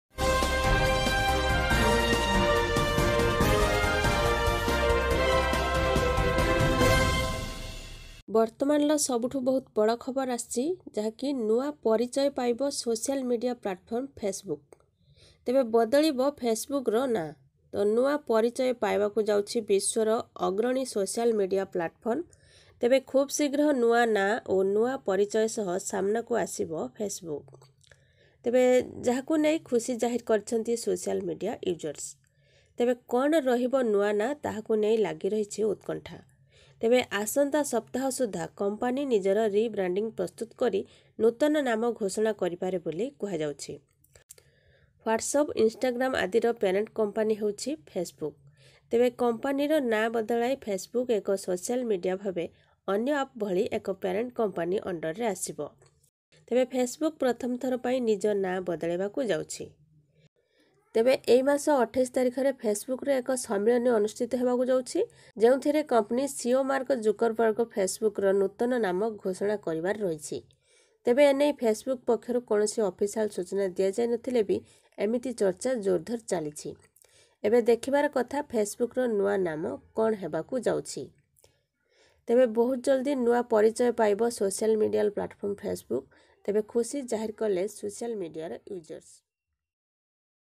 Facebook ବ୍ୟବହାର କରୁଥୁଲେ ବଡ଼ ଘୋଷଣା ,ଆଉ ଚାଲିବ ନାହିଁ ଫେସ୍ ବୁକ || Today's Breaking news| odisha
ସୋସିଆଲ ମିଡିଆରେ ଆଉ ଚାଲିବ ନାହିଁ ଫେସ୍ ବୁକ ।
Morning news
odisha
breaking news
26th October 2021